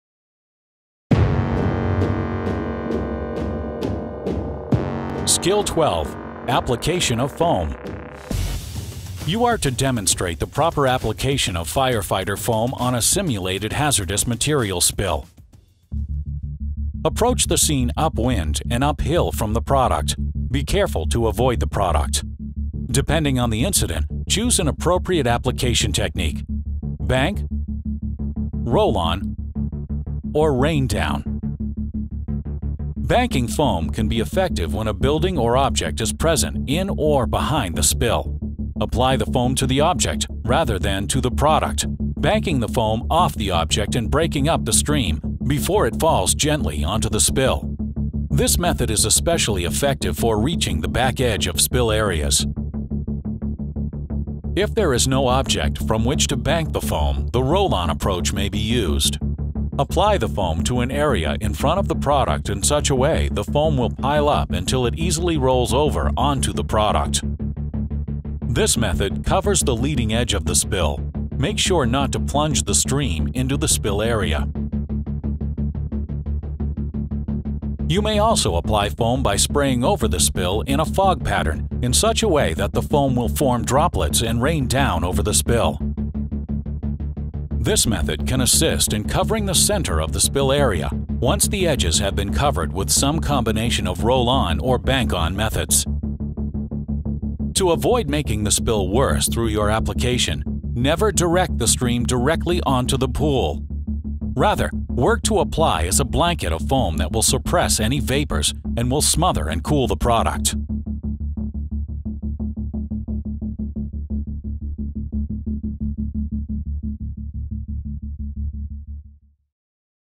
UFRA HazMat Operations Skill 12 • Applying Foam
Utah Fire Service Certification Standard for HazMat Operations Skill 12: Applying Foam
SKILL: Demonstrate the proper application of fire-fighting foam on a simulated hazardous materials spill.
REFERENCE: NFPA 1072, 2017 Edition, 6.6.1
CONDITION: Given a simulated gasoline spill (36 sq. ft.), wearing full PPE as defined by
the Emergency Response Guide, established foam application system
(apparatus or foam extinguisher), 2-member team.